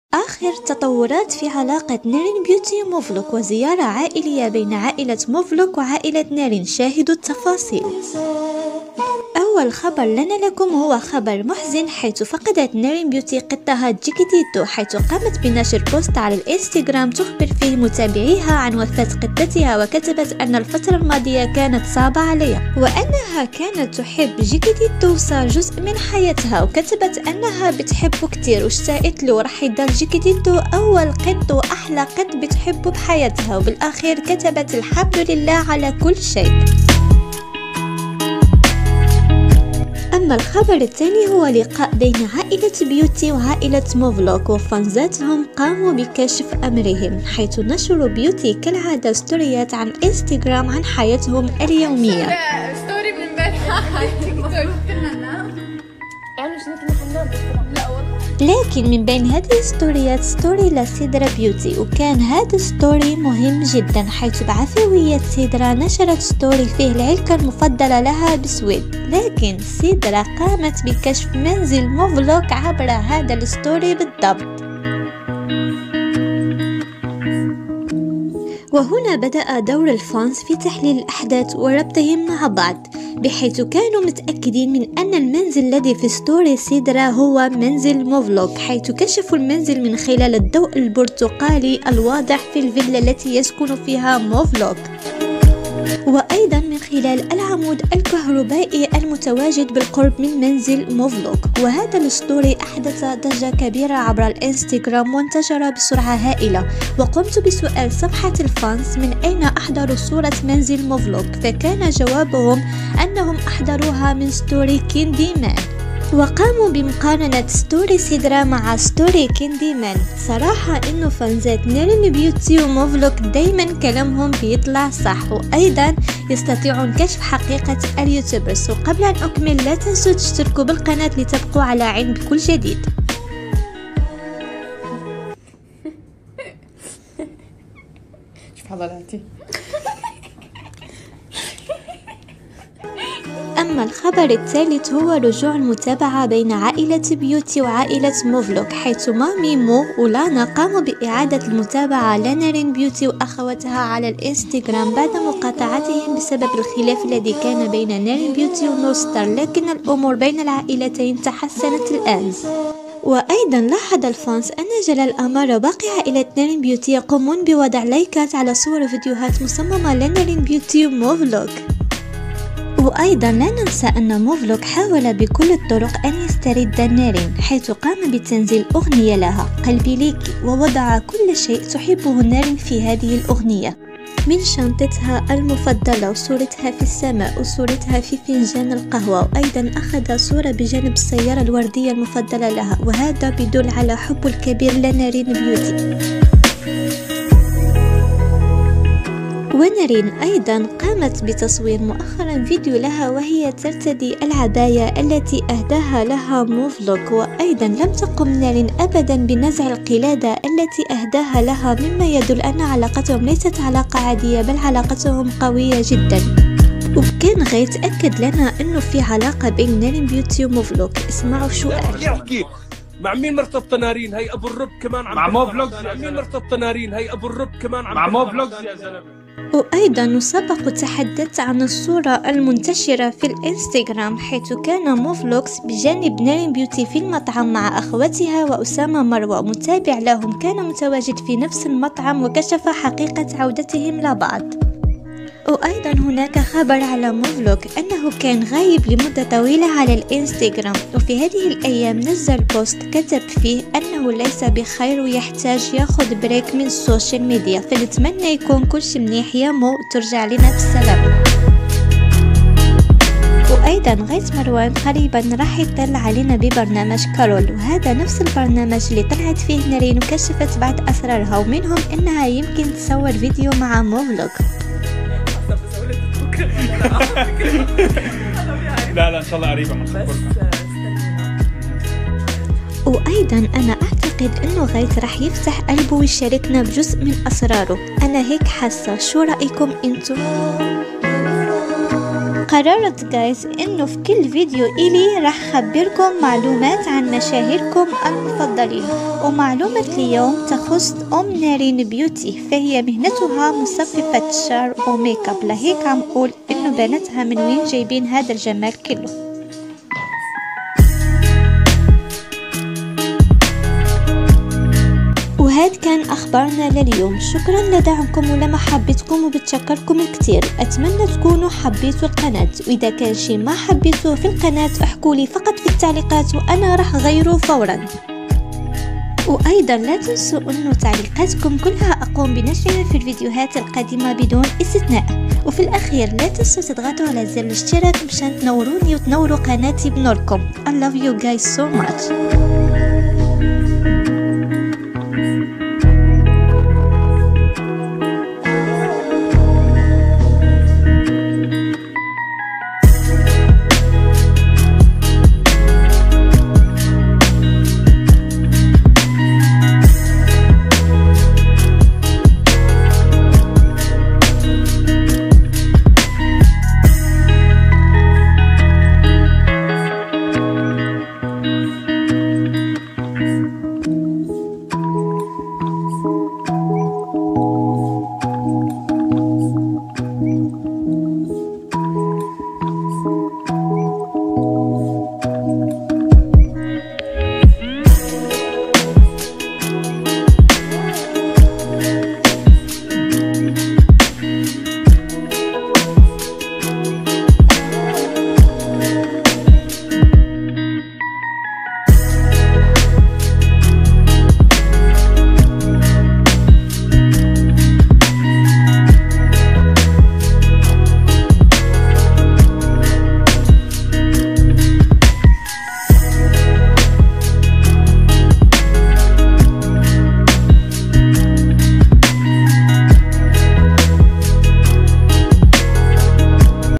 وأخيرا نارين بيوتي واخواتها في منزل مو فلوق ❤😍🔥وعودة اشهر كوبل في يوتيوب نارين ومو فلوقز ❤
سلام جايز كيفكم ❤
فيديو اليوم بحكي فيه شوي اسرار اشهر كوبل في اليوتيوب @narinsbeauty و @movlogs
وحكيت بالادلة انو اخوات بيوتي في منزل مو فلوق وكلشي بالادلة
تجدون في قناتي فيديوهات كشفت فيها اسرار نارين ومو فلوق
متابع يقوم بتصوير نارين بيوتي ومو فلوق معا
انكشفت الحقيقة نارين بتحب مو فلوق
فضيحة مو فلوق بسبب اخته

وكمان حكيت سر من اسرار @ghaithmarwan
وباقي اليوتيوبرز

قناتي @moonnewss
قناة اخبار اليوتيوبرز في الوطن العربي